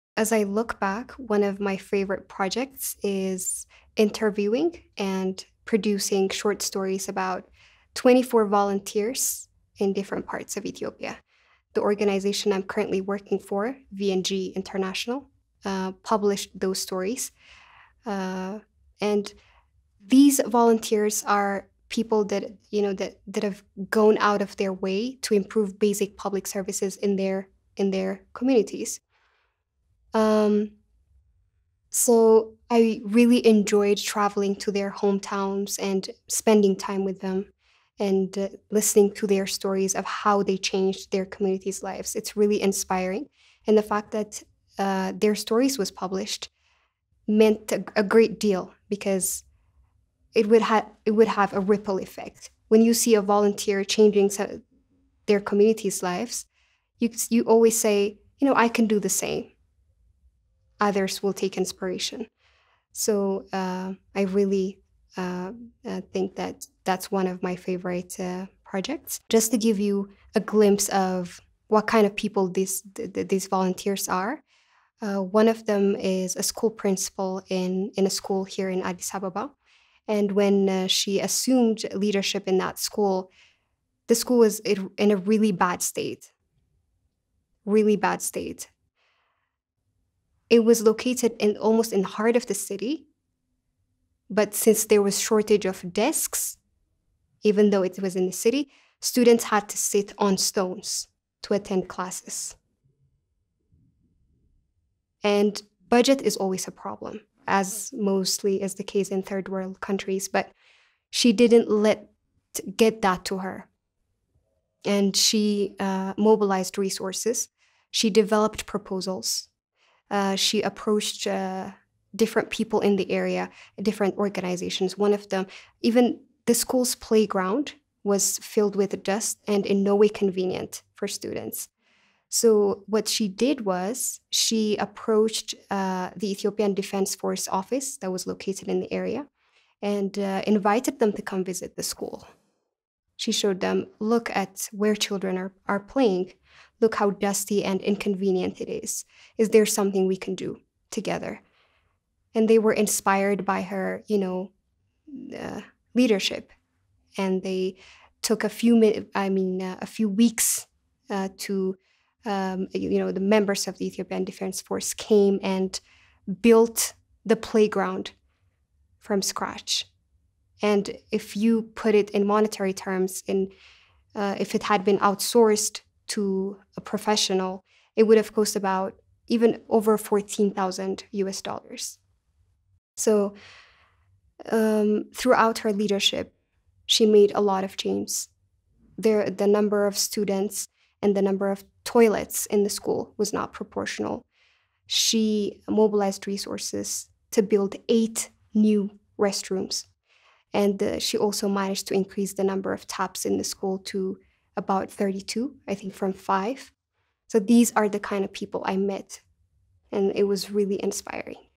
Stories About Volunteers in Ethiopia | PR and Communications Expert Serkalem Tafesse
Watch Serkalem Tafesse, a public relations and communications expert and a writer for the travel website Culture Trip. She creates content that focuses on the people, culture, and history of Ethiopia.